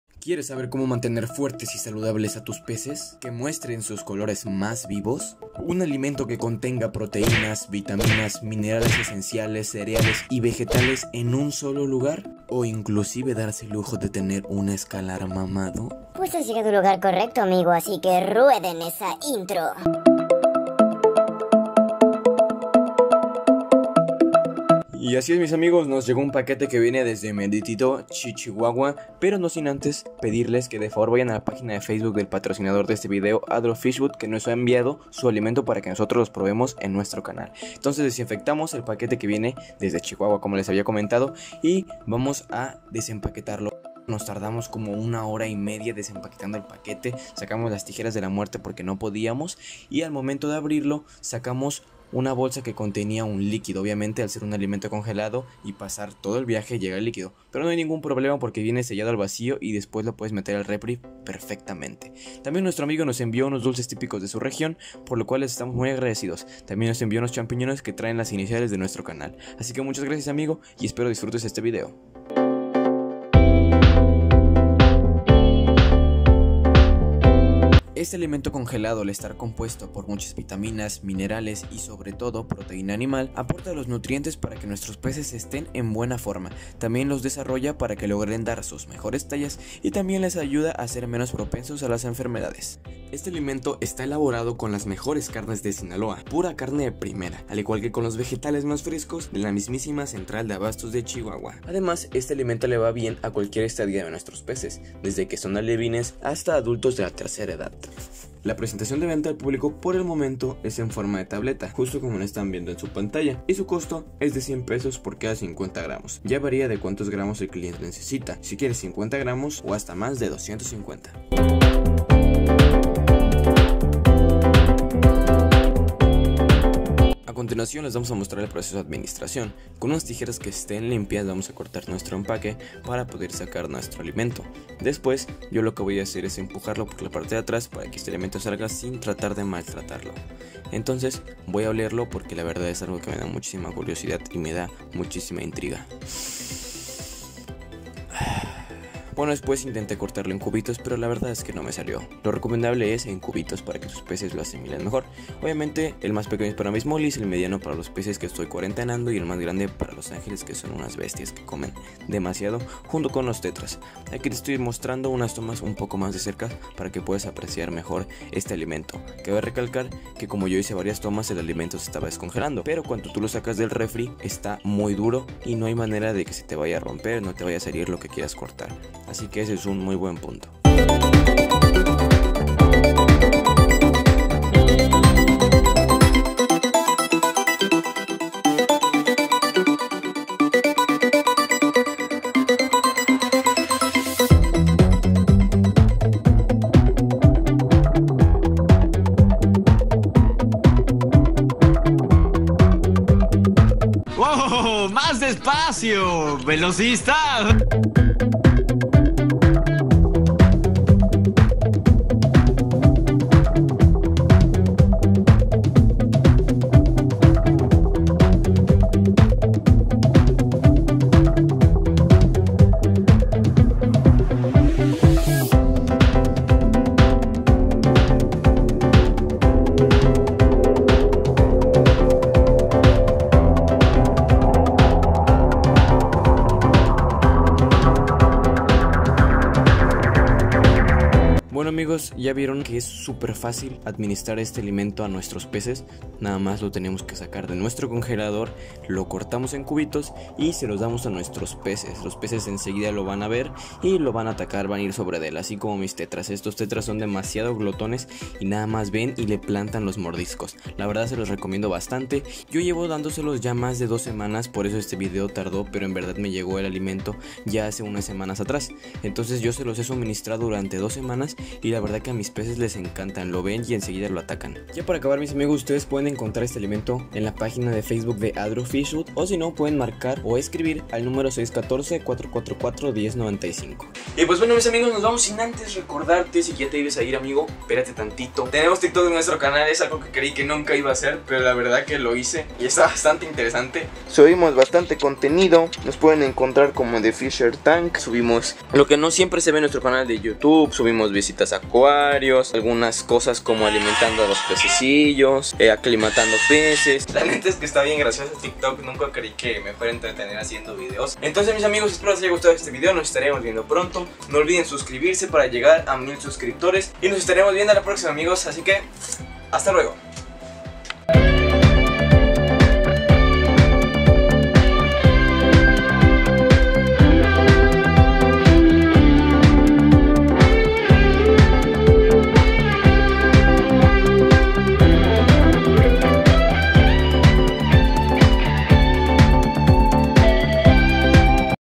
Papilla Congelada para Peces? Mejora su coloración y la salud de nuestros peces I ADRO FISHFOOD I
---  NO OLVIDES SUCRIBIRTEEEEE ---

Amigos el día de hoy hablamos sobre una papilla congelada para nuestros peces, nos sirve desde alevines hasta los mas grandes.
No olviden mencionarle que van de nuestra parte !!!

Link de la pagina de Facebook de ADRO FISHFOOD

ramirezdavid_04
TheFisherTank